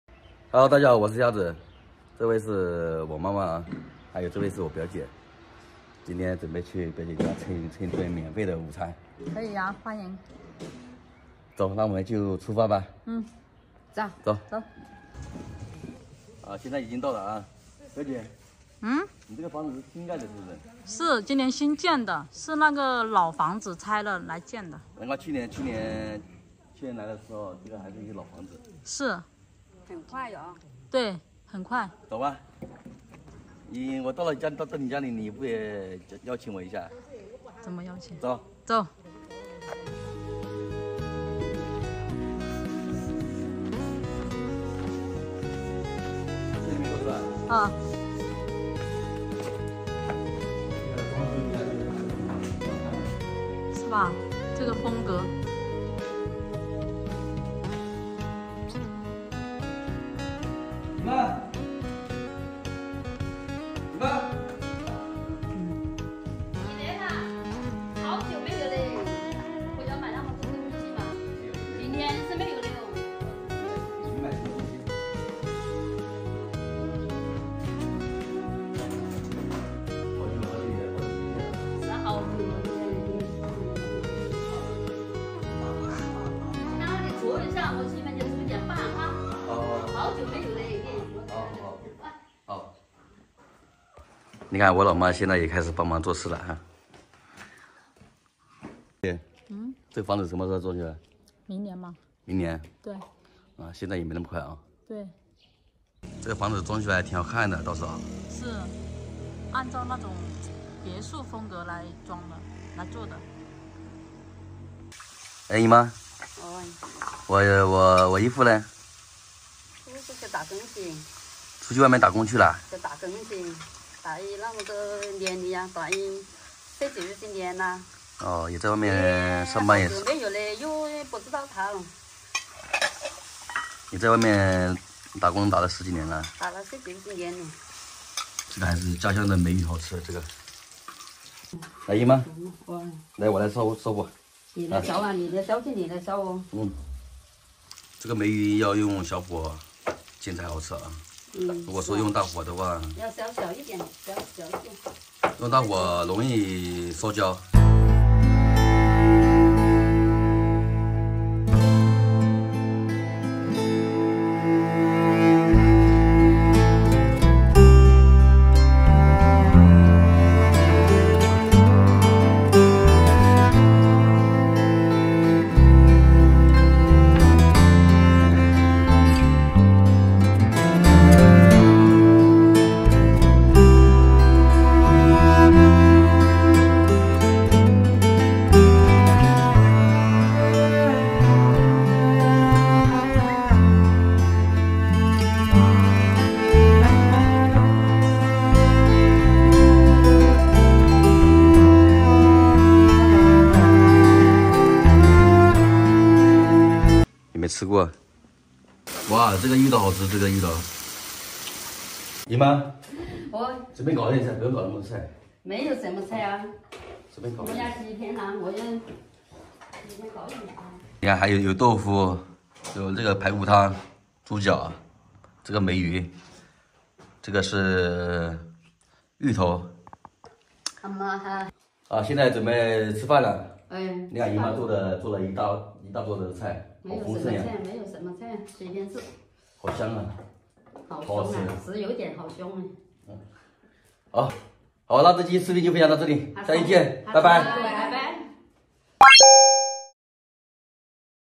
今天去姨妈家做客，姨妈说我好久没来，刚好好久没尝到姨妈做的菜
大家好，这里是【鸭子寻缘】YouTube官方唯一频道
独立摄影师，江湖行路人，生活调色家
记录和分享途中的美好